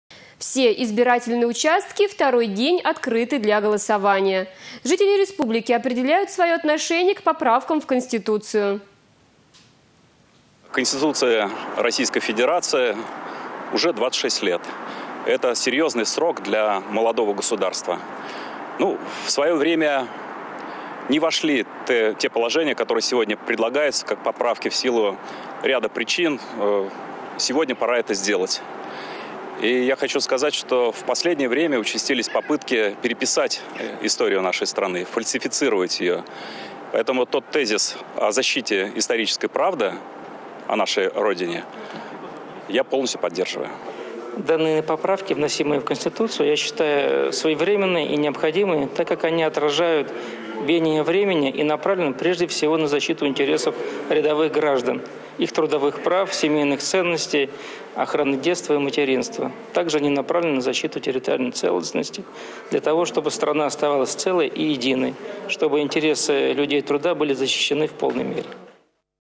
В Чувашии продолжается голосование по поправкам
Все избирательные участки сегодня открыты для голосования.  Жители республики  определяют свое отношение к поправкам в Конституцию.